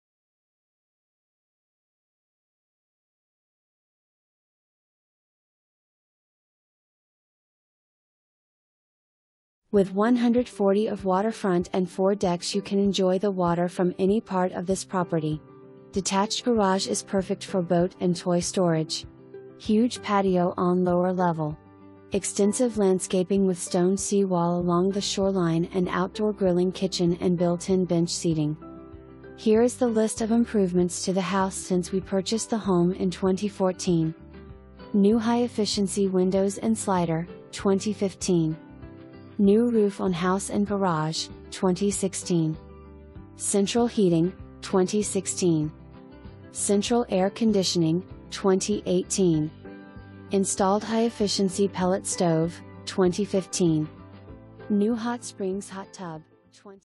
620 CLARK LOHR ROAD
620 CLARK LOHR ROAD
SWANTON, MD 21561
$985,000, 3 bed, 2.00 bath, 2,112 SF, MLS# MDGA133360

with 140' of waterfront and four decks you can enjoy the water from any part of this property. Detached garage is perfect for boat & toy storage. huge patio on lower level. Extensive landscaping with stone sea wall along the shoreline and outdoor grilling kitchen and built-in bench seating.
Here is the list of improvements to the house since we purchased the home in 2014:
-New high efficiency windows and slider (2015)
-New roof on house and garage (2016)
-Central heating (2016)
-Central air conditioning (2018)
-Installed high efficiency pellet stove (2015)
-New Hot Springs hot tub (2018)
-New hot water heater (2020)

2 full bath

Dale Carpenter
Railey Realty